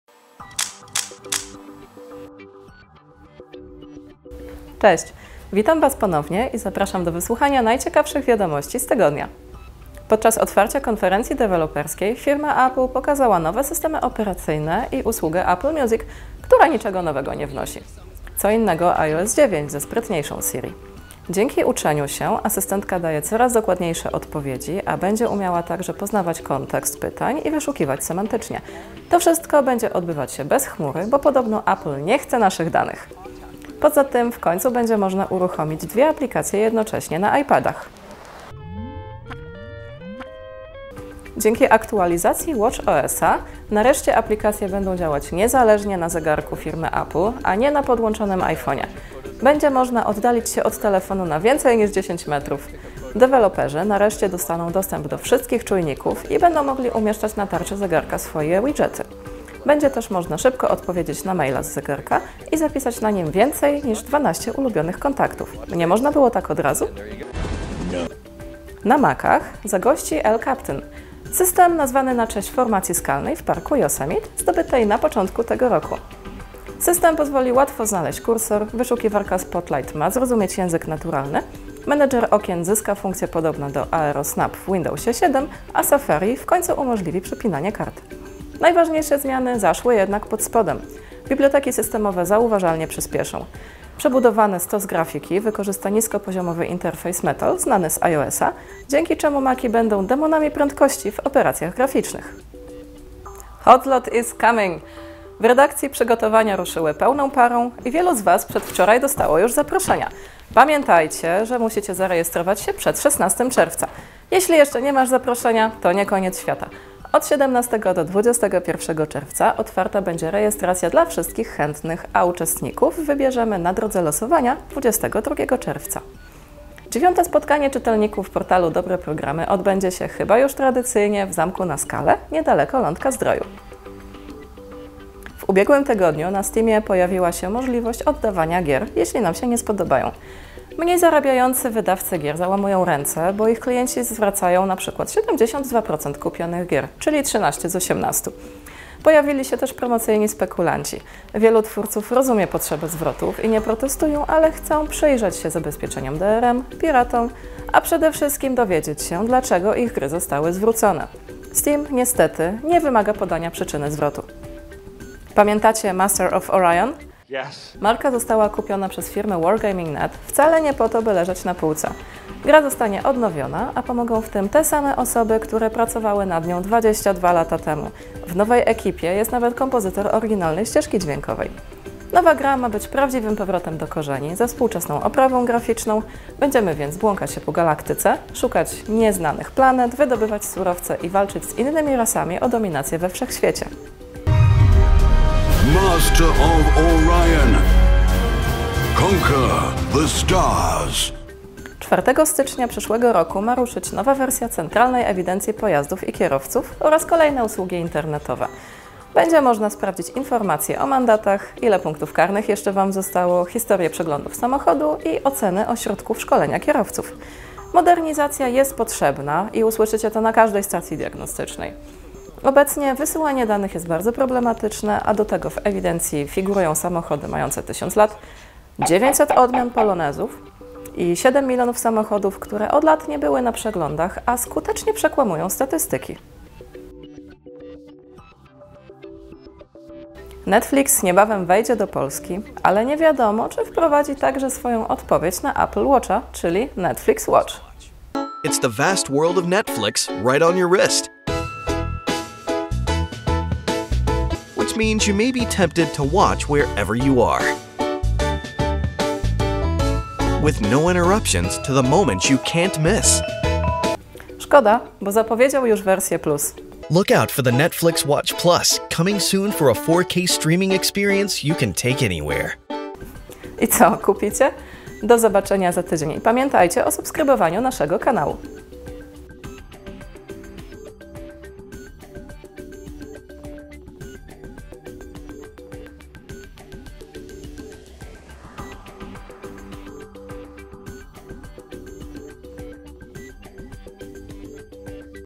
Snapshot 12.06.2015 – tydzień w nowych technologiach
Nie wiesz co się działo? Nie miałeś czasu czytać? Obejrzyj dziesiąty odcinek naszego piątkowego programu informacyjnego.